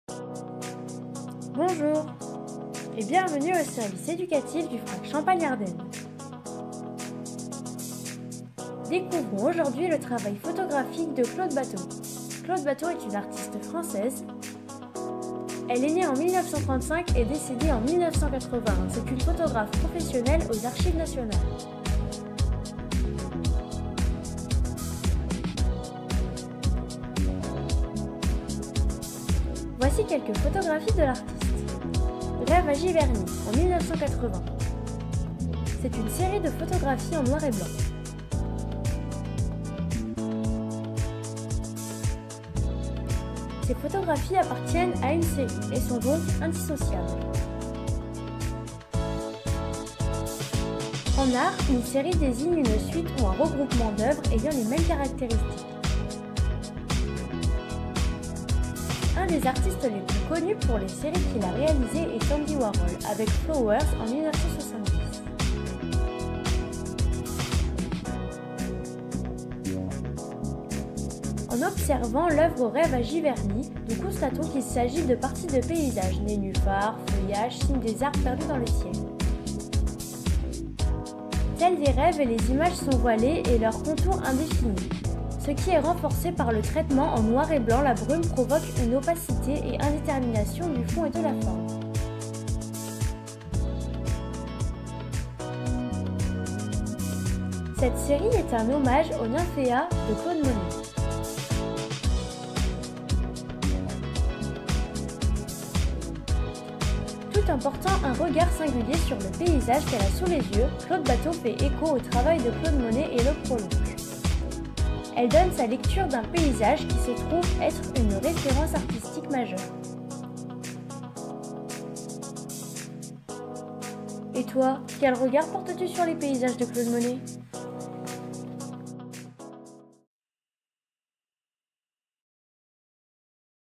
"Rêve à Giverny" de Claude BATHO.
Le service éducatif du FRAC champagne-ardenne vous propose de découvrir une œuvre de la collection : "Rêve à Giverny" de Claude BATHO.